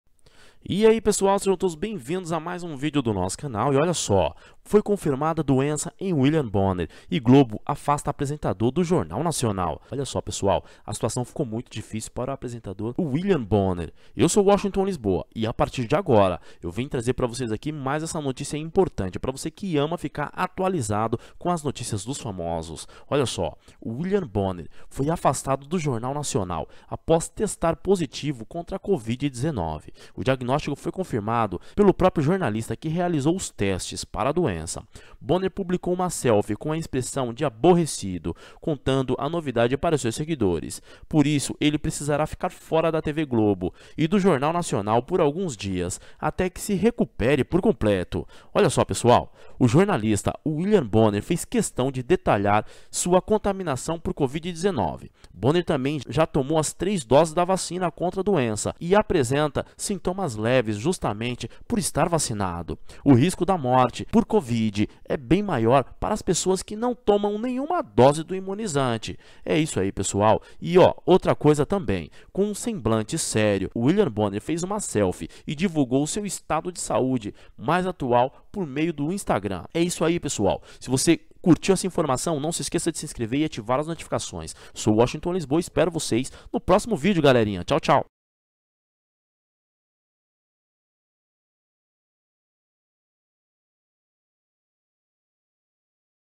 URGENTE! Foi confirmada D0ENÇA em William Bonner, e Globo afasta apresentador do Jornal Nacional
Foi confirmada doença em William Bonner, e Globo afasta apresentador do Jornal Nacional

"-Tchau,brigadooo!"